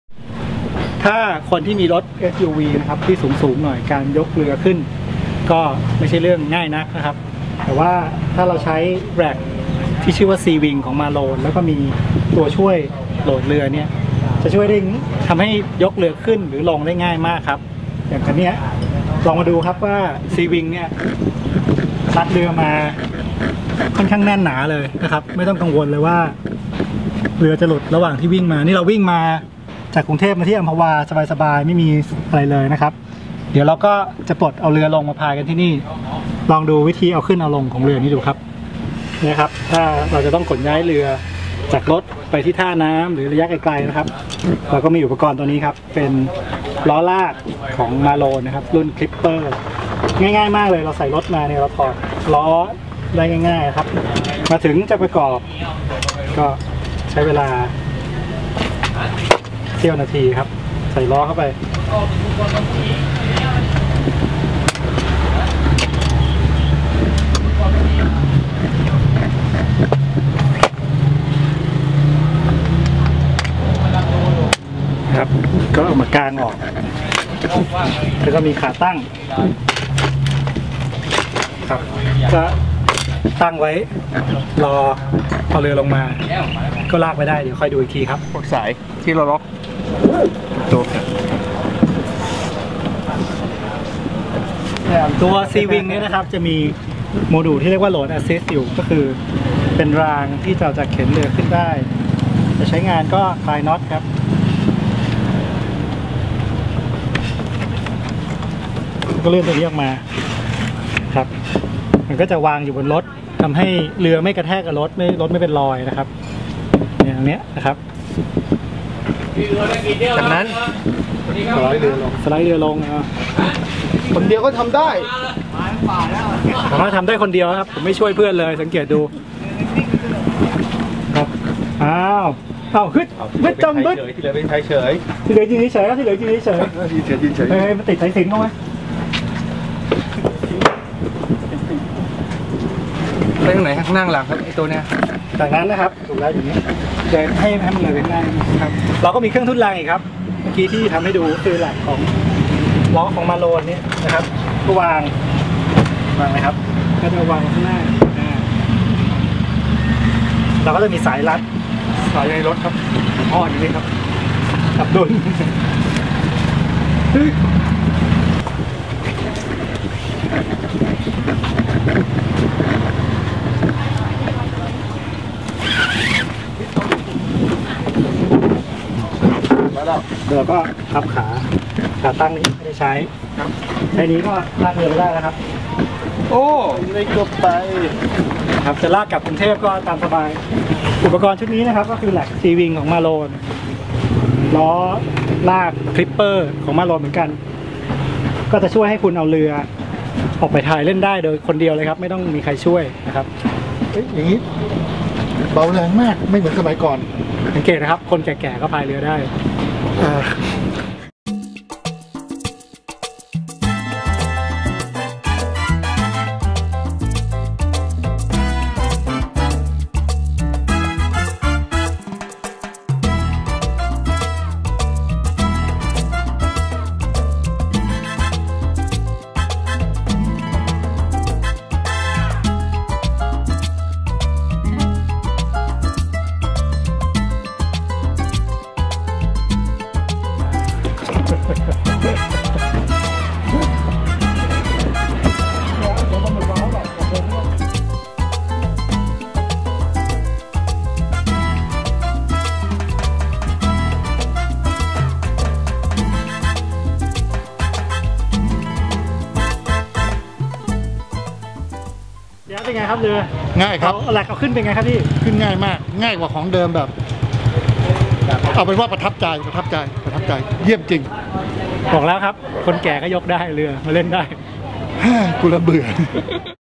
Malone Seawing
อุปกรณ์ขนย้ายเรือ Rack รุ่น SeaWing และล้อลาก Cipper ที่จะช่วยให้การขนย้ายเรือออกไปเล่นเป็นเรื่องง่ายๆ